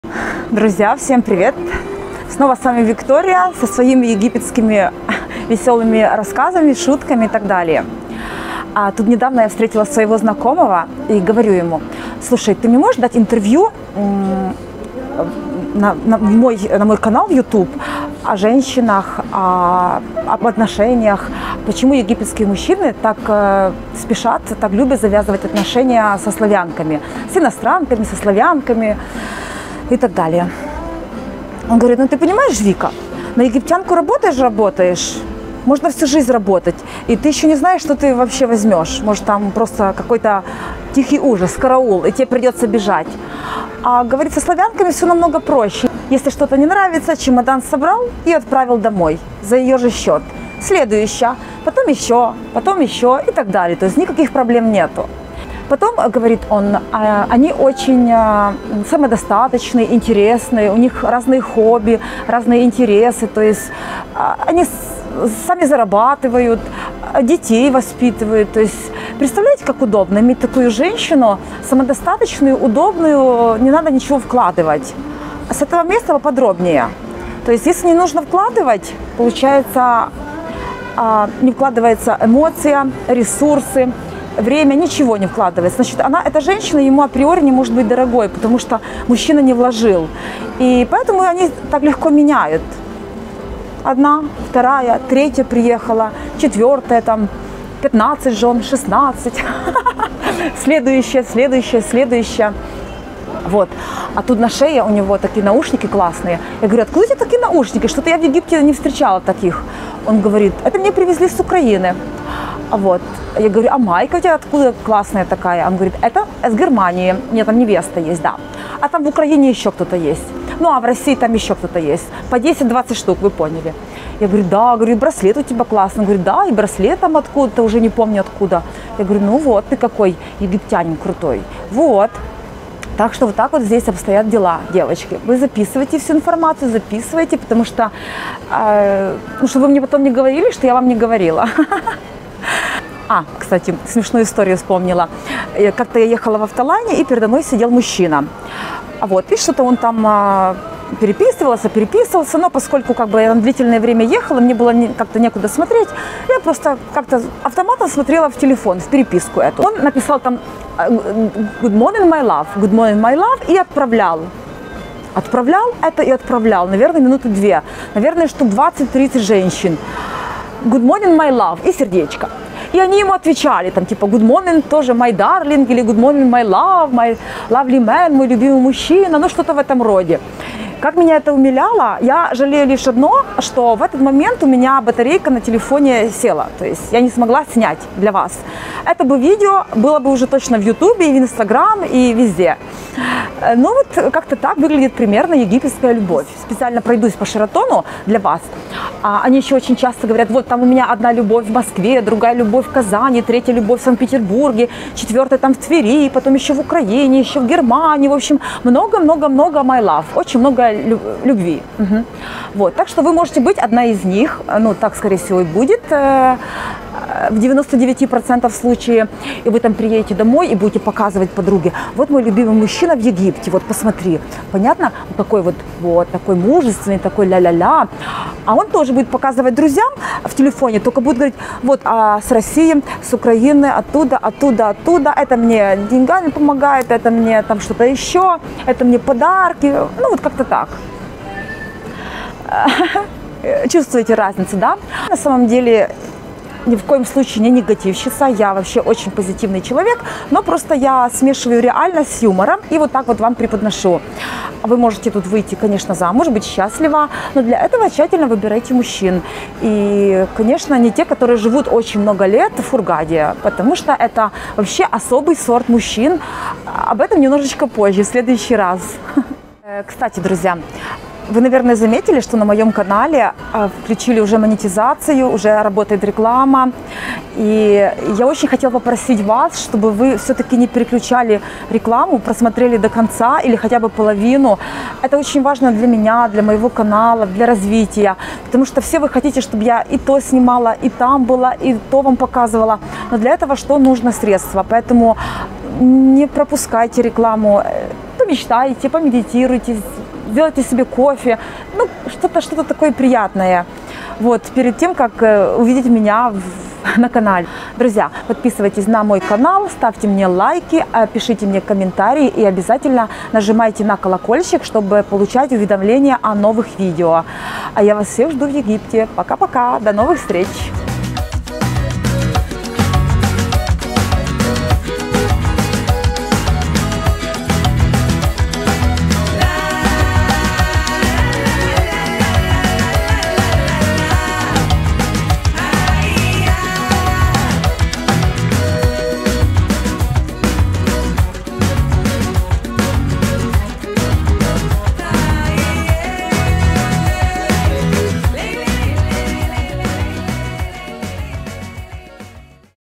ПОЧЕМУ ЕГИПТЯНЕ ВЫБИРАЮТ РУССКИХ 👬🏾жизнь в Египте🌴ЕГИПЕТСКИЕ "ЖЕНИХИ"💓(funny time)
От себя лично скажу, что в Египте очень много хороших и добрых мужчин, я знаю точно.Но, проблема в том , что в туристический город едут не очень порядочные люди, а другая проблема в том - что тот , кто хочет быть ЖЕРТВОЙ , то они в любой стране будут ЖЕРТВАМИ!

А я люблю Египет и уважаю Египетских мужчин, потому что у меня очень много порядочных и хороших друзей♥️

On my own behalf, I’ll say that there are a lot of good and kind men in Egypt, I know for sure. But, the problem is that not very decent people go to the tourist city, and another problem is that the one who wants to be a Victim, then  they will be VICTIMS in any country!

Помогу приобрести Недвижимость в Египте🏠